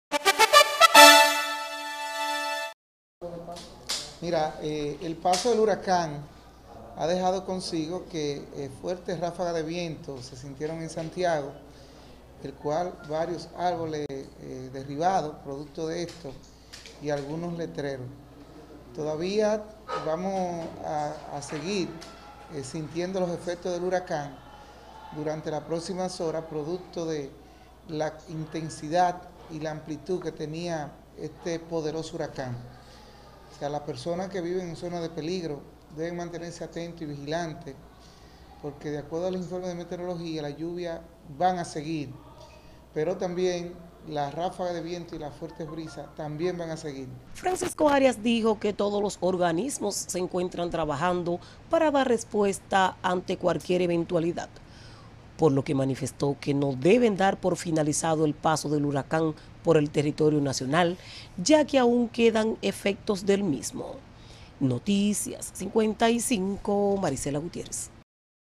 Defensa Civil se mantiene vigilante antes los efectos Matthew .-
DIARIO55, SANTIAGO.- El director regional de la Defensa Civil, Francisco Arias, llamó a la población a mantenerse vigilantes ante el incremento de las lluvias que puedan caer en las próximas horas en esta ciudad y la región Norte, por lo que alerta a la ciudadanía estar atenta a la evolución del fenómeno Matthew y a las informaciones de los organismos autorizados.-